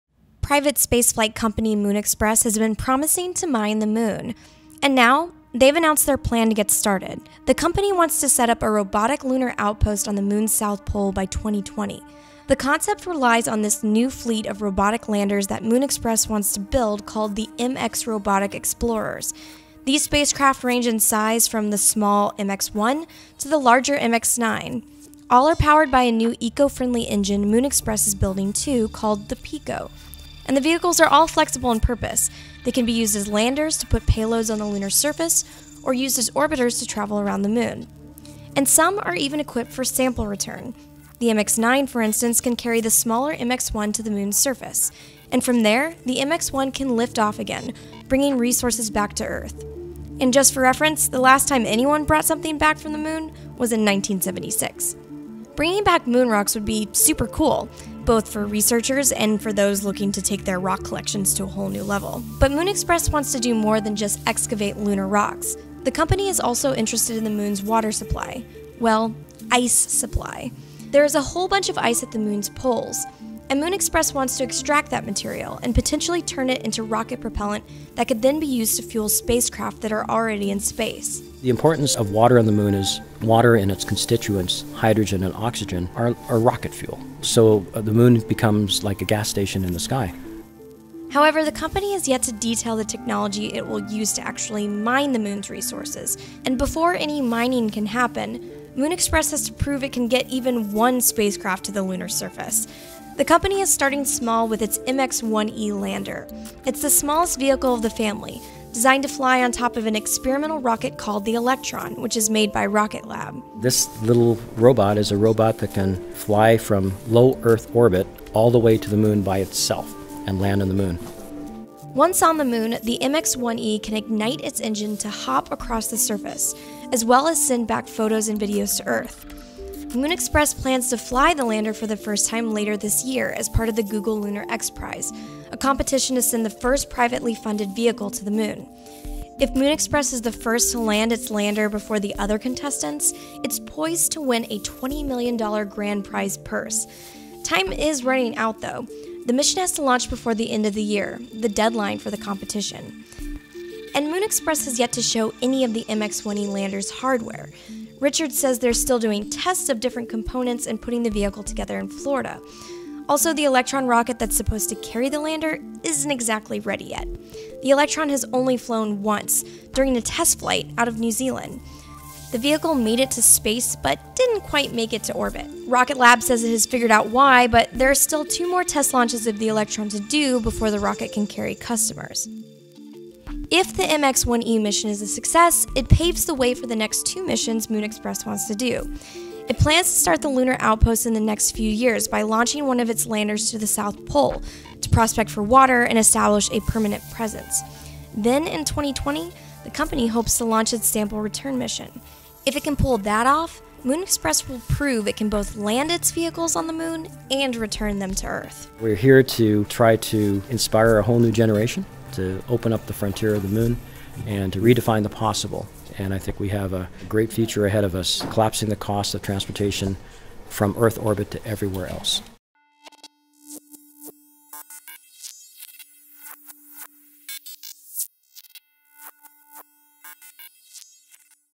How Moon Express could use robots to mine the Moon
Today, private spaceflight company Moon Express unveiled its grand plans to build a robotic outpost on the Moon as early as 2020. The ultimate goal is to mine for water and minerals — resources that can then be sold for profit.